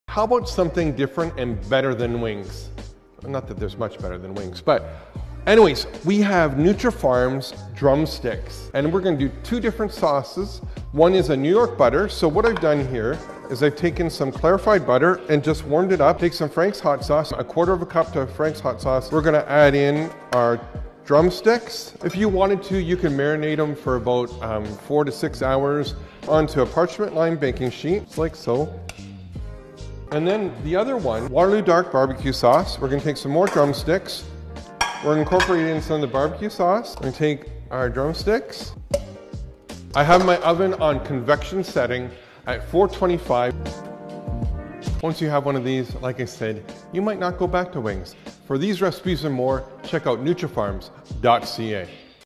BBQ Sauces - Nutrafarms
2 Awesome BBQ Sauces for Drumsticks courtesy of @chefdtv 🤤

Simple and simply delicious!

For more delicious recipes, visit the link in our bio!